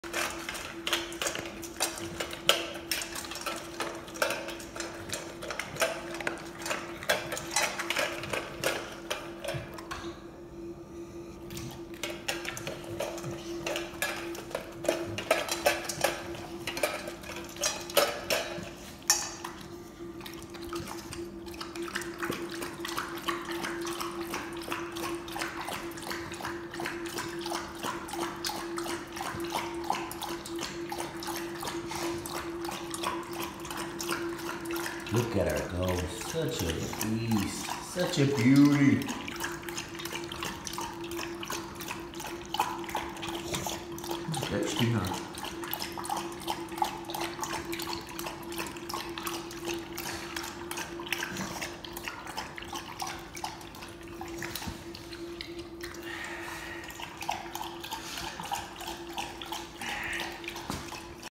The beast is hungry lol
Watching my big girl eat...